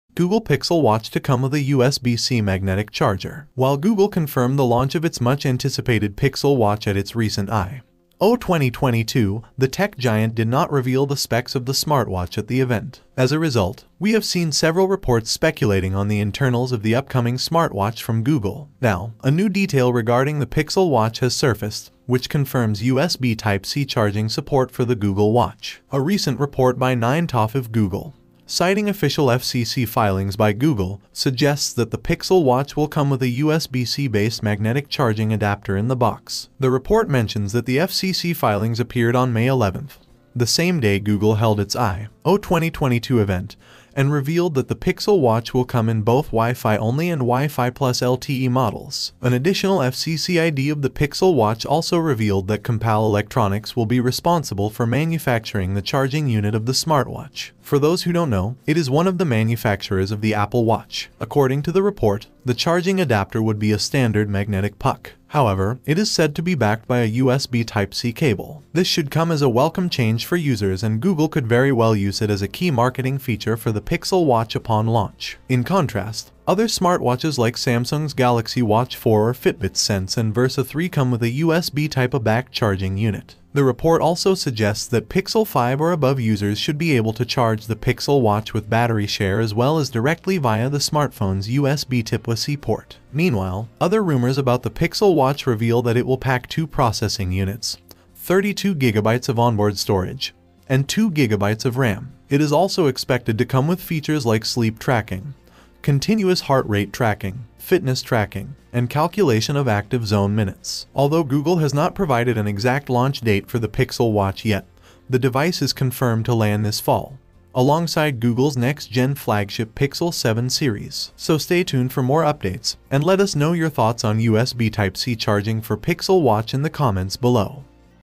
Google pixel watch : Google Pixel Watch to Come with a USB-C Magnetic Charger.
While Google confirmed the launch of its much-anticipated Pixel Watch at its recent I/O 2022, the tech giant did not reveal the specs of the smartwatch at the event. As a result, we have seen several reports speculating on the internals of the upcoming smartwatch from Google. Now, a new detail regarding the Pixel Watch has surfaced, which confirms USB-C charging support for the Google watch.

For those who don’t know, it is one of the manufacturers of the Apple Watch. According to the report, the charging adapter would be a standard magnetic puck. However, it is said to be backed by a USB-C cable.